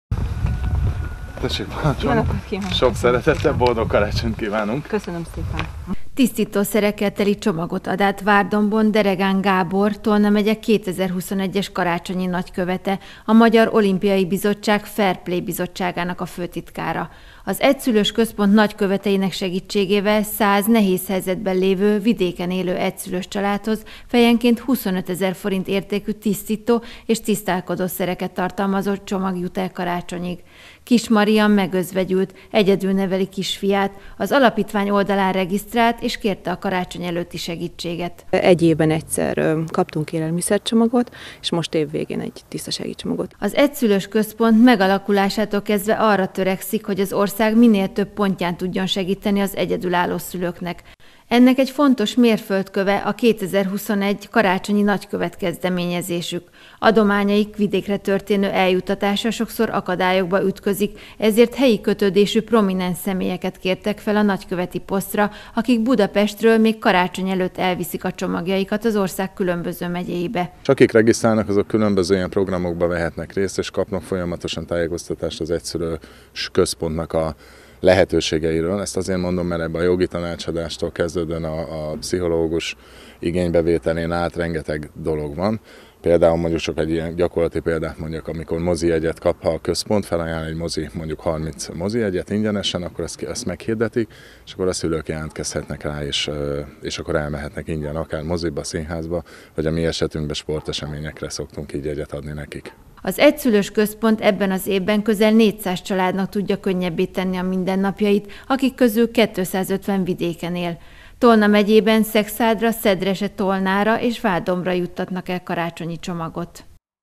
Karácsonyi nagykövet adta át az ajándékot - az egyszülős családokat támogatták
Huszonötezer forint értékben kapott öt Tolna megyei településen élő egyszülős család tisztítószer csomagot a budapesti székhelyű Egyszülős Központ jóvoltából. A Karácsonyi Nagykövet kezdeményezésnek a keretein belül prominens személyek száz, vidéken élő rászoruló családnak juttatják el az adományt az ünnepek előtt.